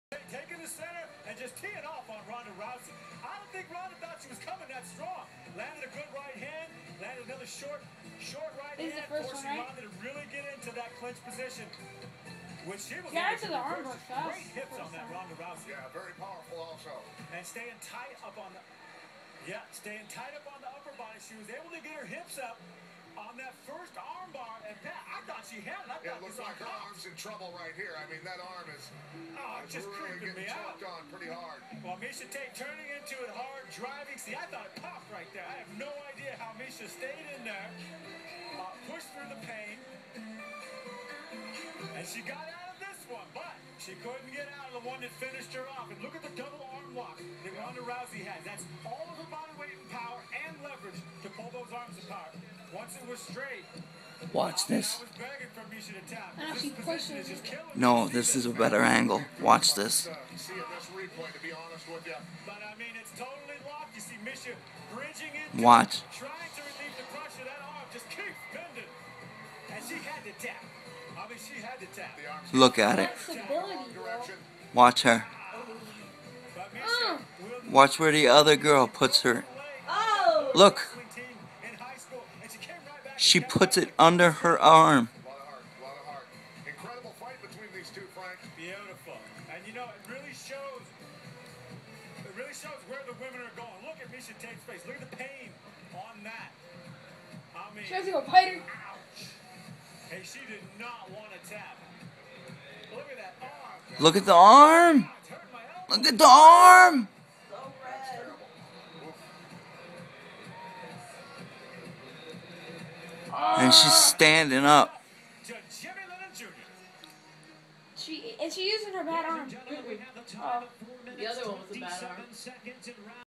Tate vs Rousey
Woman's MMA, Strikeforse on Showtime. Rousey puts Tate in an arm bar and hyper-extends her elbow to a 90 degree angle.